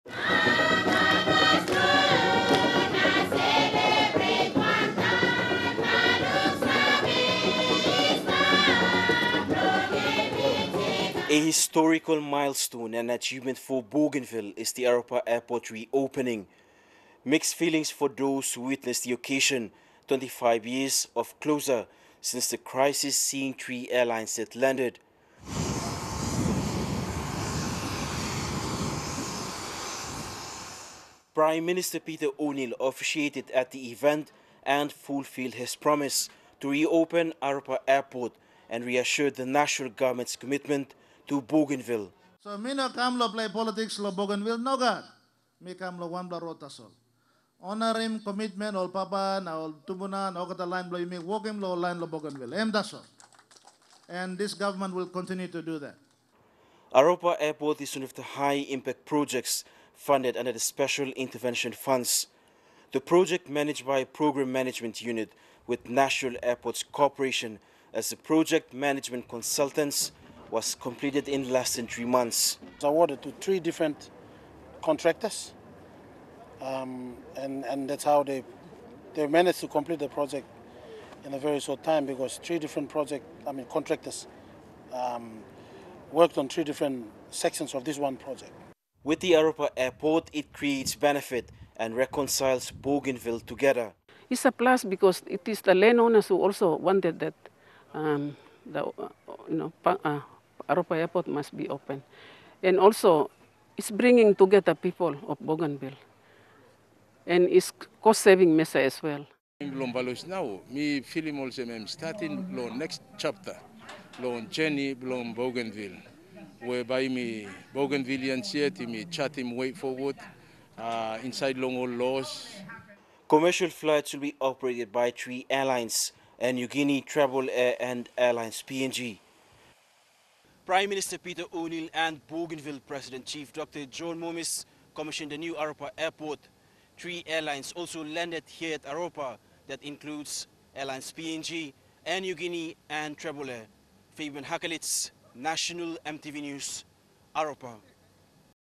Aropa Airport reopens in Bougainville
The reopening of the Aropa Airport in Kieta, Central Bougainville is a historical milestone and achievement for the Autonomous Region of Bougainville - visit us for the latest news...